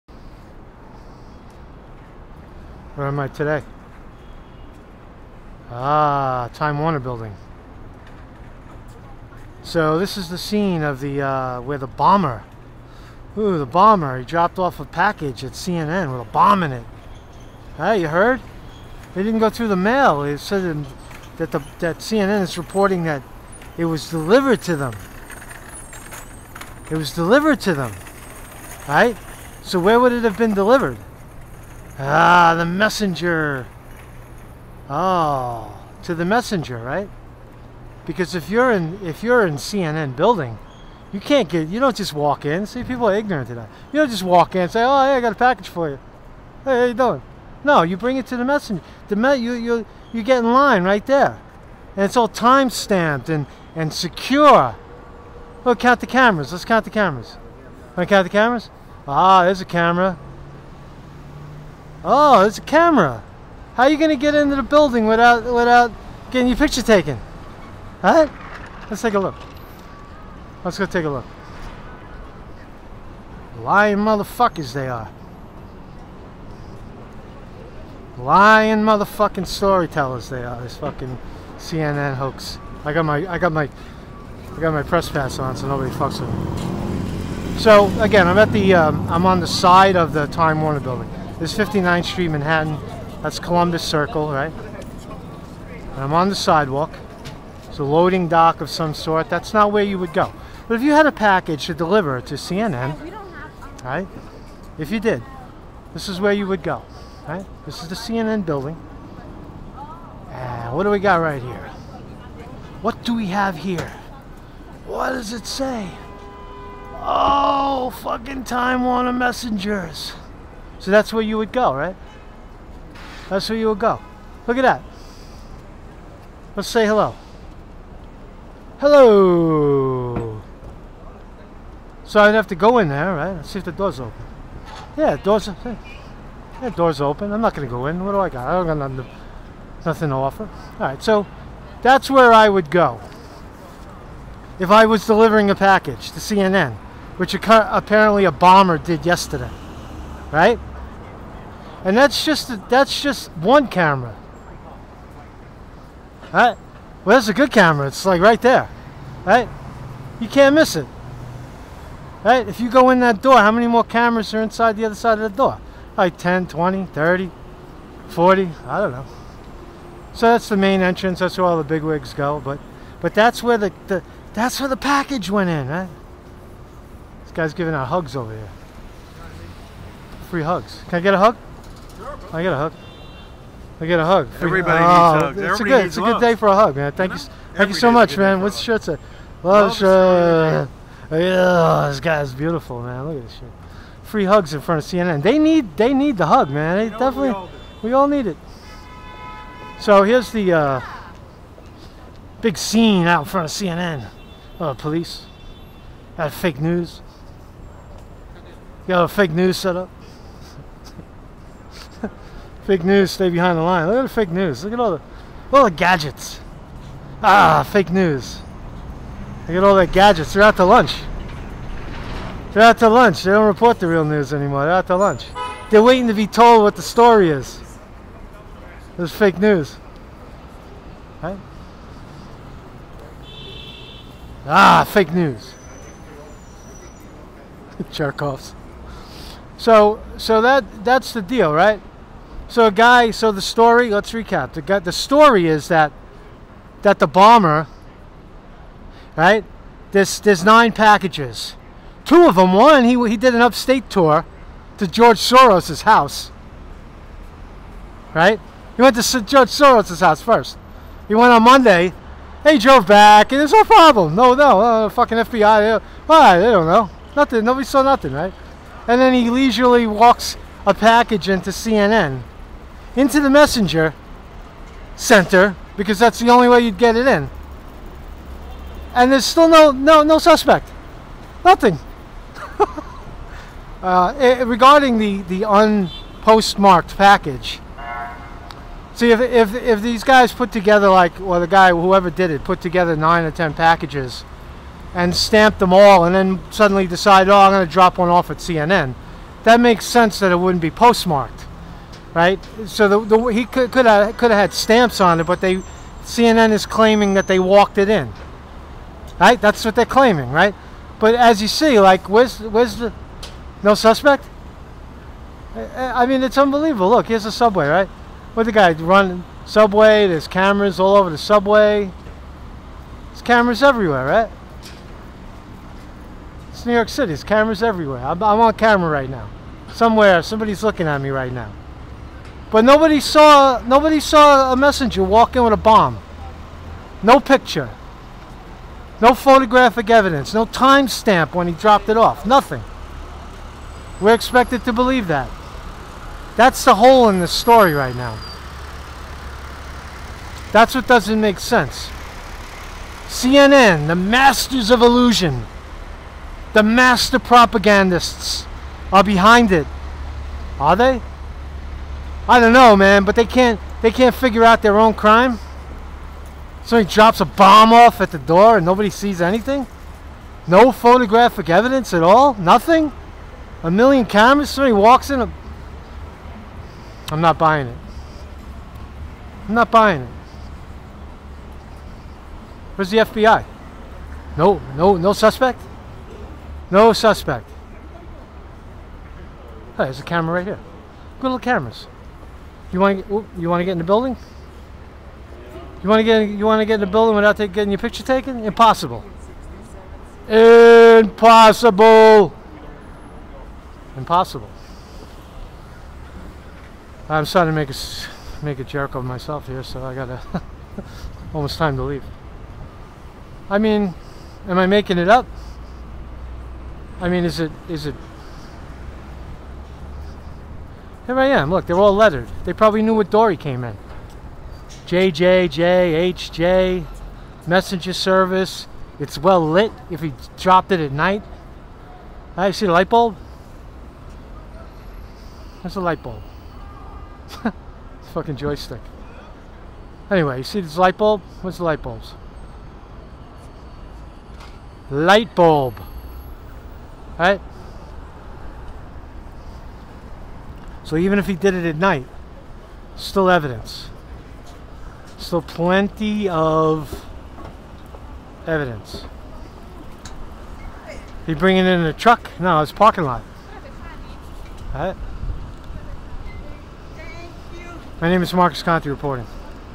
CNN Complicit In FAKE BOMB Narrative; Cameras Everywhere, No Picture Of Suspect
(3" x 3" Stickers)

Marcus Conte for United States Senate for New York:
- 100% Election Integrity